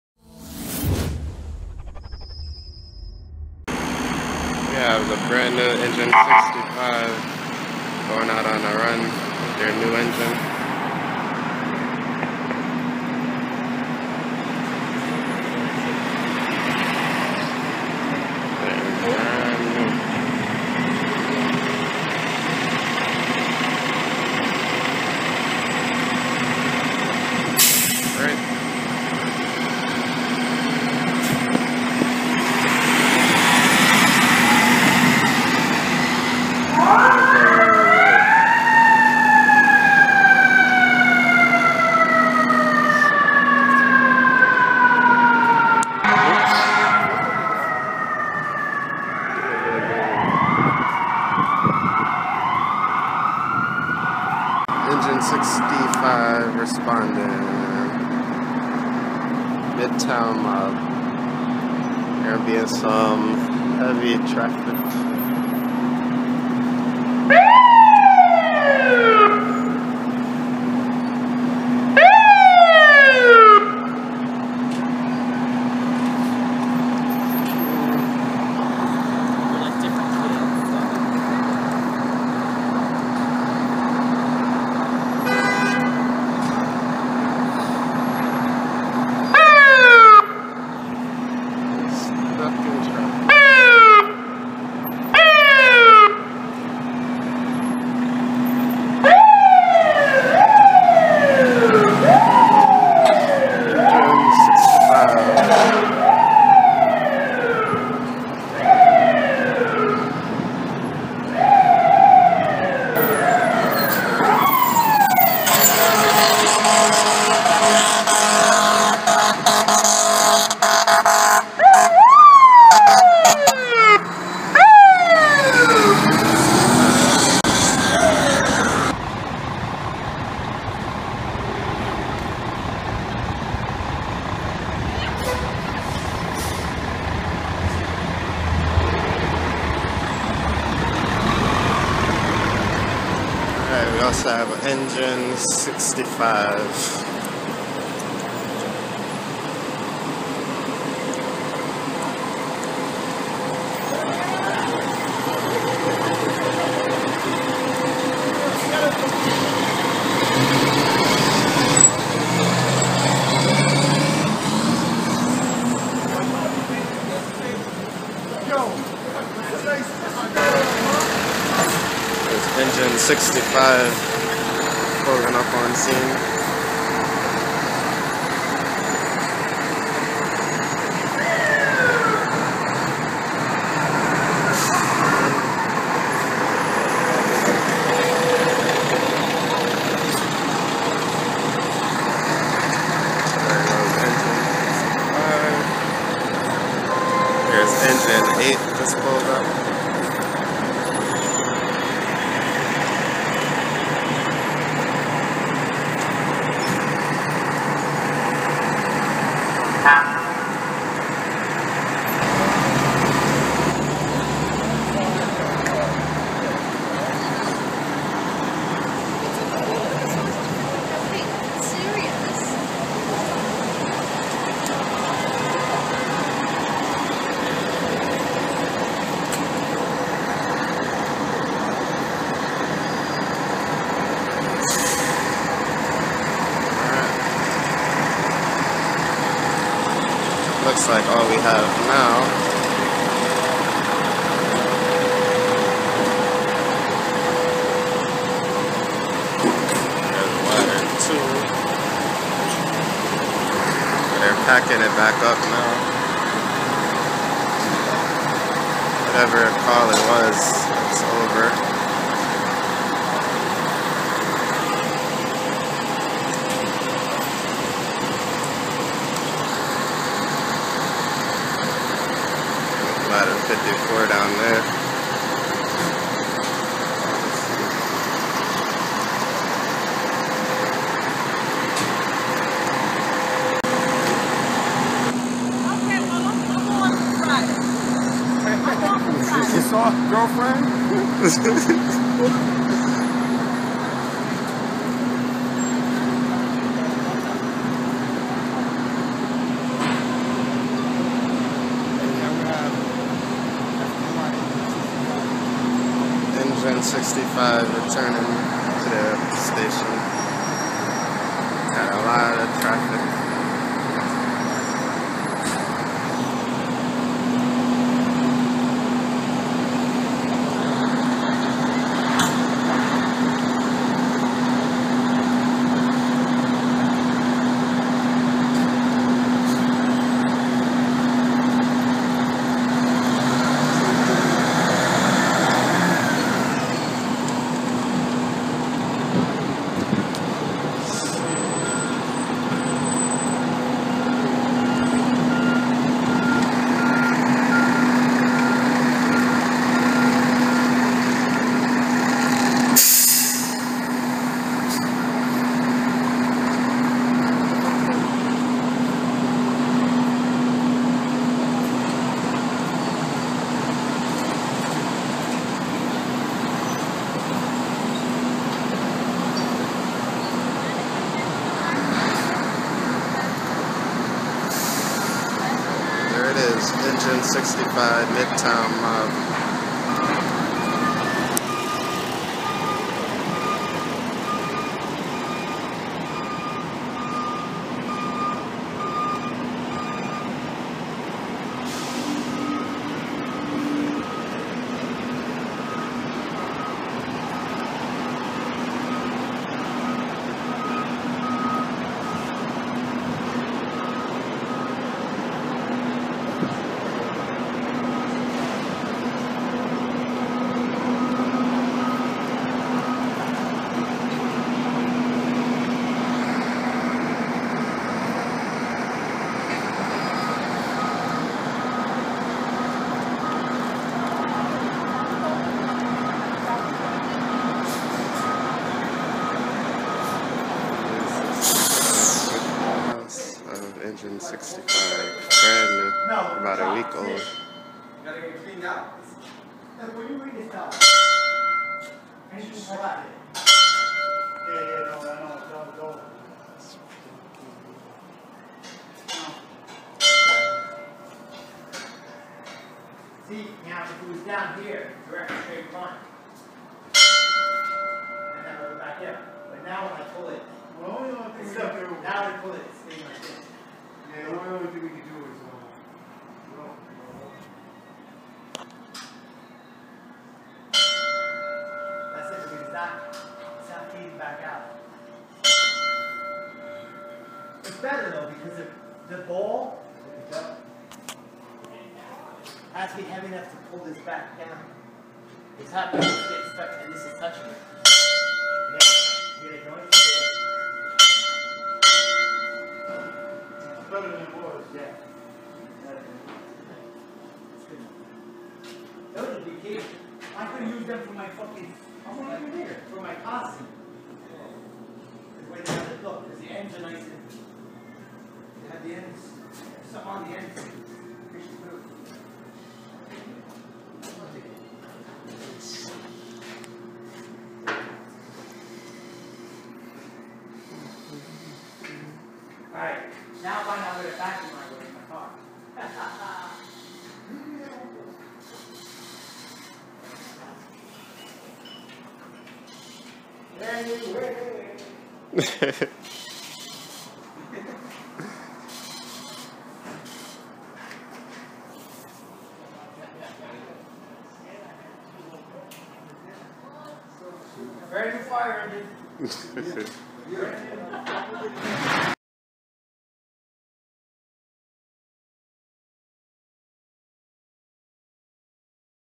Compilation Of FDNY Engine 65 Responding & On Scene Of Various Scenes Throughout Midtown Manhattan
New York City Fire Department - FDNY Engine 65 "The Midtown Mob" compilation. This video includes several clips I have of Engine 65 responding, in house, and on scene of various calls throughout Manhattan.

Thanks!!!

Major Thanks to John AKA THEMAJESTIRIUM1
Be sure to check out his amazing videos.